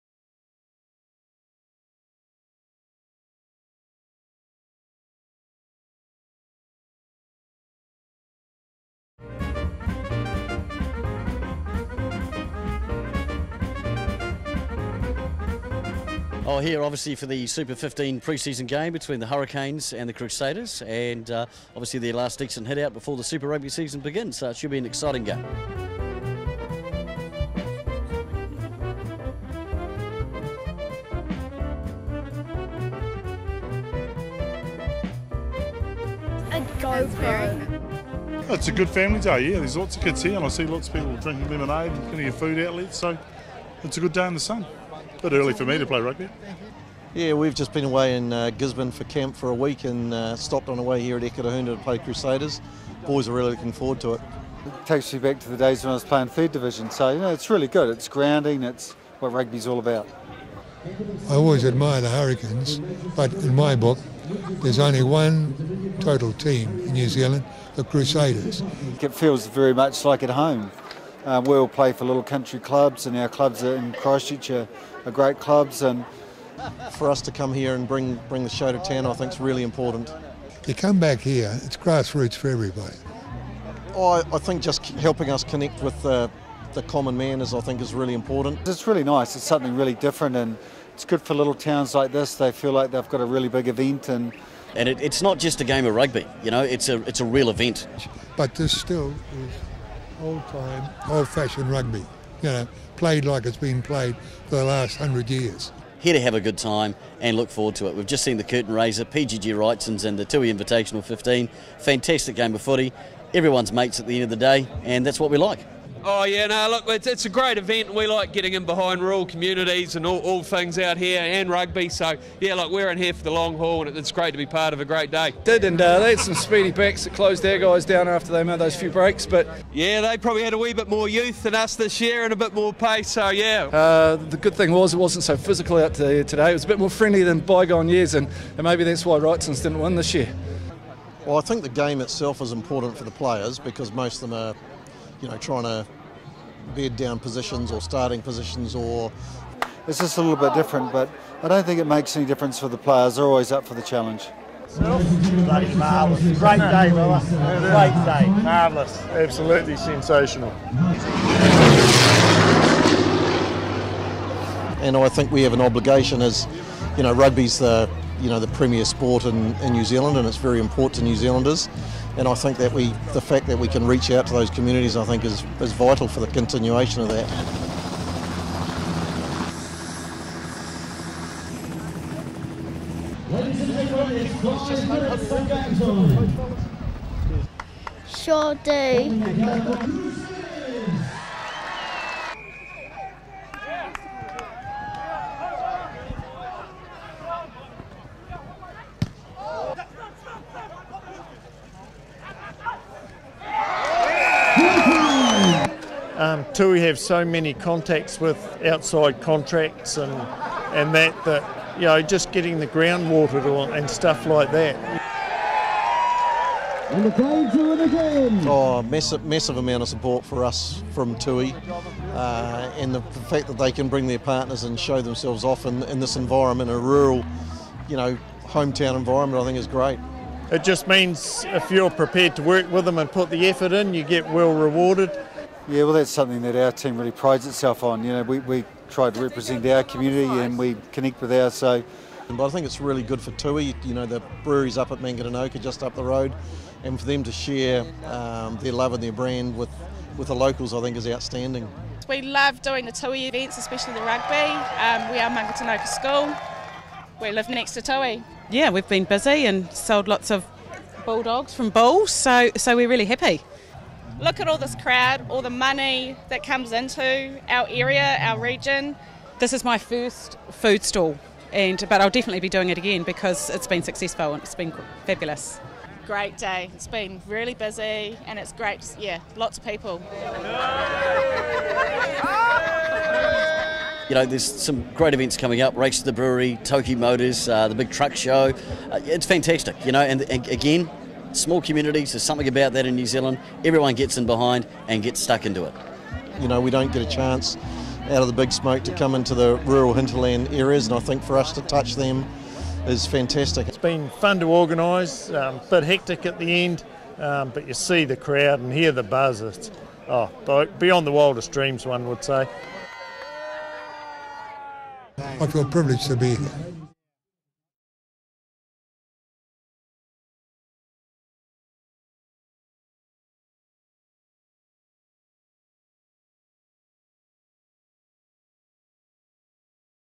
Tui HQ 2015 Pre-season Rugby
Tui HQ preseason rugby between The Hurricanes and The Crusaders is an event you can enjoy.

This story is told by the people who worked to be there on the day and acknowledges those who support the regional rugby and business communities.

Produced by Deane from Actual Vision, based in South Wairarapa.